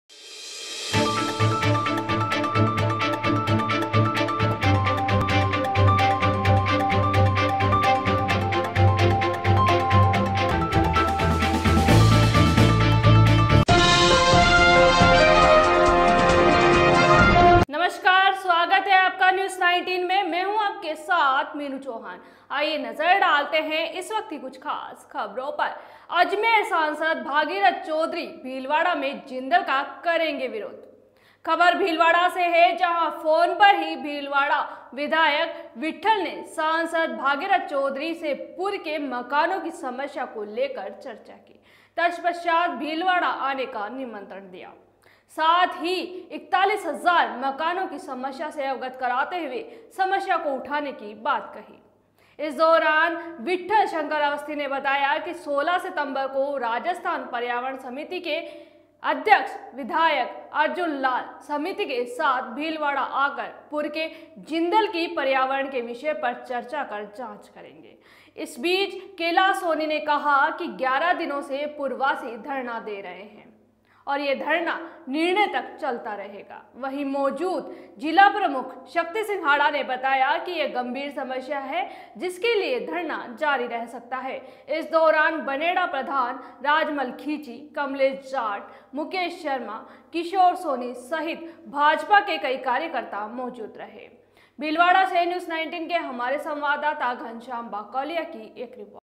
भीलवाड़ा
अजमेर सांसद भागीरथ चौधरी आज भीलवाड़ा में,जिंदल का विरोध करेंगे